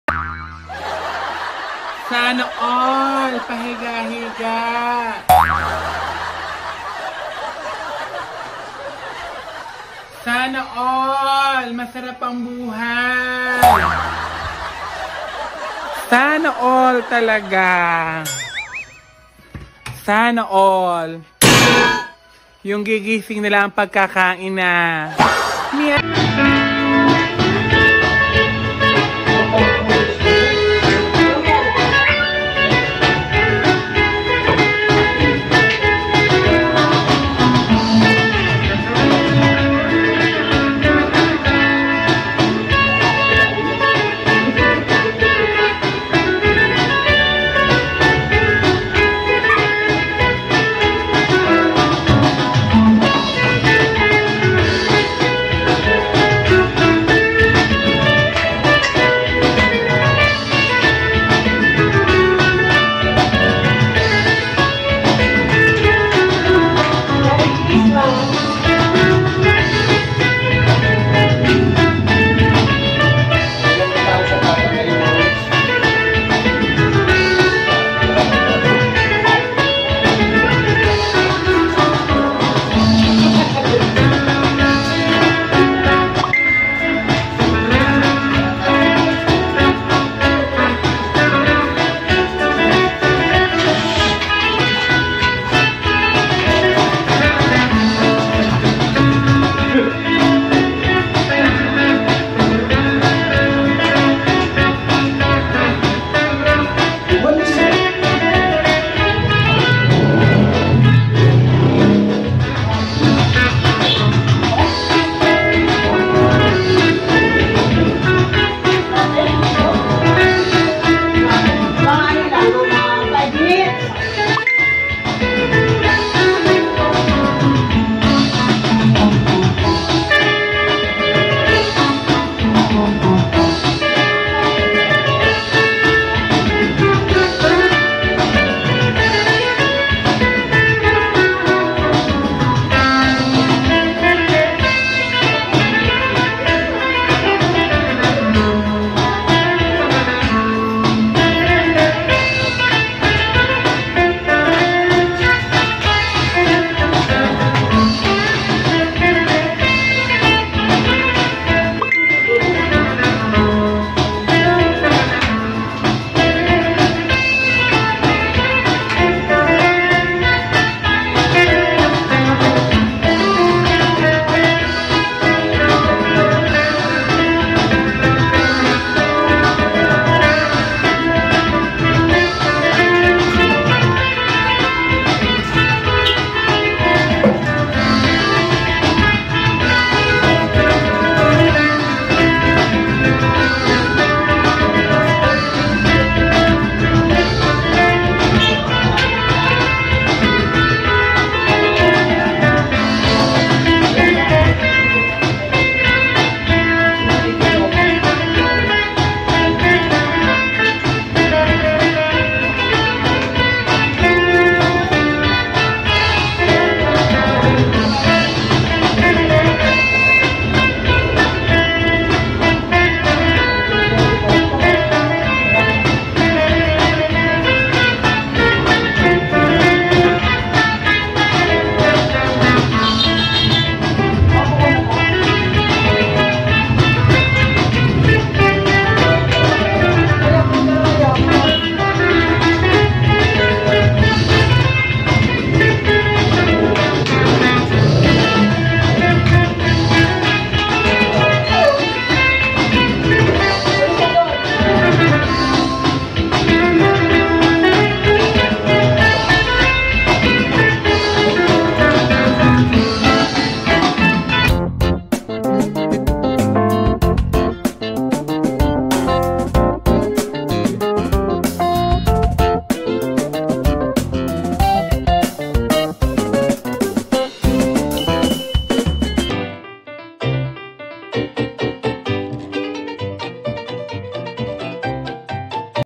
HINDI AKO TAMAD. NAGPAPAHINGA LANG AKO
Walang taong tamad. Sadyang may mga pagkakataon na mas masarap matulog at magpahinga lalo na kung malamig ang panahon. @Wowa 818 TV